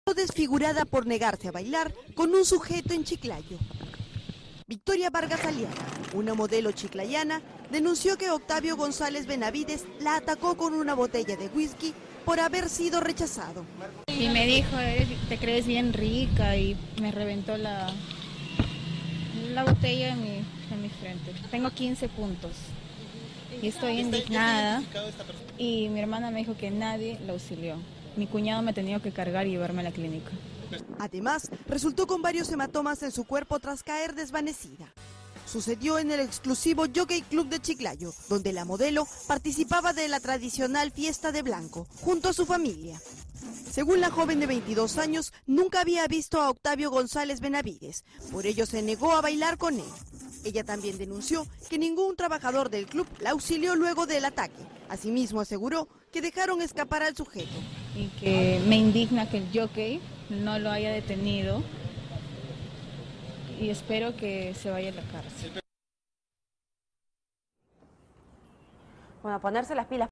Chiclayo: sujeto desfigura rostro a joven que se negó a bailar con él en una fiesta (2/2)
1

Victoria Vargas Aliaga es la joven que resultó gravemente herida luego de ser golpeada con una botella de whisky en el rostro, cuando se encontraba en una fiesta organizada por el Jockey Club de Chiclayo.

La joven de 22 años, que terminó con severos hematomas en la frente, acusó a Octavio Gonzales Benavides de haberla agredido salvajemente luego de rechazar su invitación a bailar.

Pese a su estado, la víctima acudió a la comisaría de La Victoria para denunciar al agresor y pedir su intervención ya que pudo costarle la vida.

2